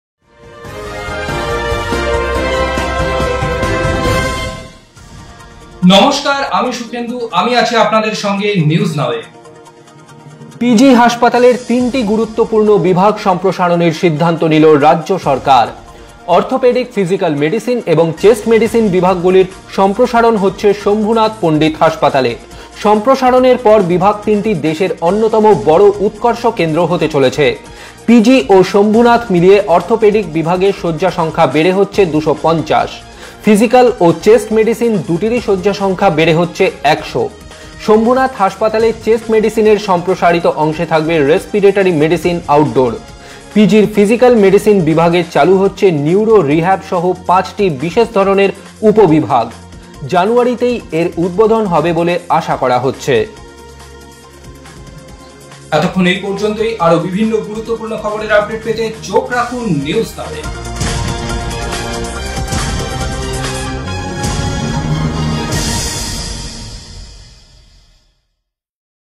পি জি হাসপাতালের তিনটি বিভাগের সম্প্রসারণ, দেশের অন্যতম উৎকর্ষ কেন্দ্র হতে চলেছে
Three departments in PG Hospital being upgraded, to become among the best in the country

WATCH